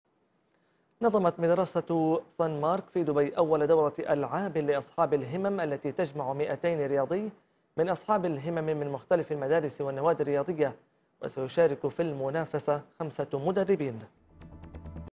دبي: تنظيم فعالية انفينتي الرياضية لأصحاب الهمم
نظمت مدرسة صن مارك في دبي أول دورة ألعاب انفينتي لأصحاب الهمم التي تجمع مئتان رياضي من أصحاب الهمم من مختلف المدارس والنوادي الرياضية.

وتتضمن المنافسة التي ستقام يوم الجمعة سلسلة من السباقات وسباقات الحواجز ومسابقات الجمباز.

وصرح مؤسس المبادرة أن هذه المبادرة أو مسابقة لأصحاب الهمم تتيح الفرصة للمشاركة من مختلف الجنسيات.

وصرحت هولي ميرفي ، مؤسس شركة PE  ومديرةأكاديمية Sports Heroes لأصحاب الهمم أنها دعن أكثر من 65 مدرسة للمشاركة في المنطقة للمشاركة للحدث خاصةً أصحاب الهمم من الأطفال.

وسيشارك في المنافسة 5 مدربين منتدبين من لجنة الألعاب الأولمبية الخاصة 2019 في أبوظبي، وشارك فريق الأولمبياد الخاص في تنظيم هذا الحدث بانضمام 150 رياضي منهم.

وصرح طلال الهاشمي ، المدير الوطني لدورة الألعاب الأولمبية الخاصة الإماراتية ، إن التعاون مع ألعاب انفينيتي يساعدهم على تحقيق أهم أهدافهم في إدماج أصحاب الهمم في المجتمع.